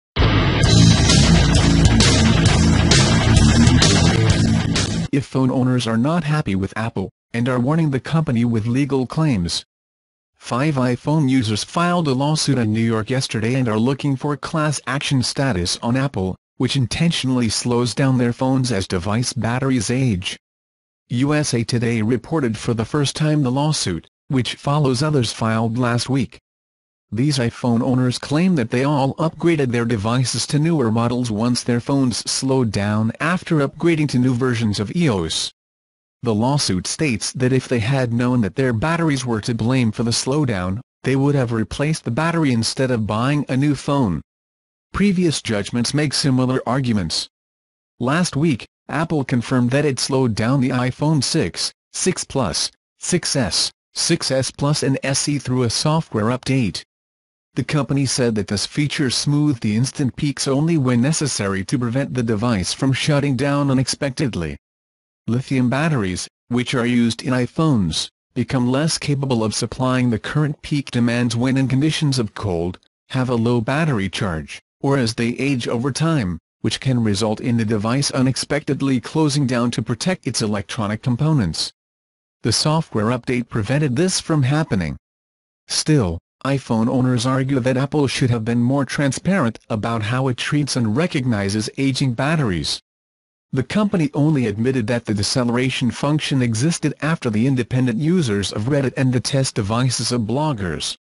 Apple faces multiple demands: after admitting it slows iPhones as its batteries age.
IPhone owners are not happy with Apple, and are warning the company with legal claims. Five iPhone users filed a lawsuit in New York yesterday and are looking for class action status on Apple, which intentionally slows down their phones as device batteries age. USA Today reported for the first time the lawsuit, which follows others filed last week.

These iPhone owners claim that they all upgraded their devices to newer models once their phones slowed down after upgrading to new versions of iOS. The lawsuit states that if they had known that their batteries were to blame for the slowdown, they would have replaced the battery instead of buying a new phone. Previous judgments make similar arguments.

Last week, Apple confirmed that it slowed down the iPhone 6, 6 Plus, 6S, 6S Plus and SE through a software update. The company said that this feature smoothed the instant peaks only when necessary to prevent the device from shutting down unexpectedly. "Lithium batteries, which are used in iPhones, become less capable of supplying the current peak demands when in conditions of cold, have a low battery charge, or as they age over time, which can result in the device unexpectedly closing down to protect its electronic components. " The software update prevented this from happening.

Still, iPhone owners argue that Apple should have been more transparent about how it treats and recognizes aging batteries. The company only admitted that the deceleration function existed after the independent users of Reddit and the test devices of bloggers.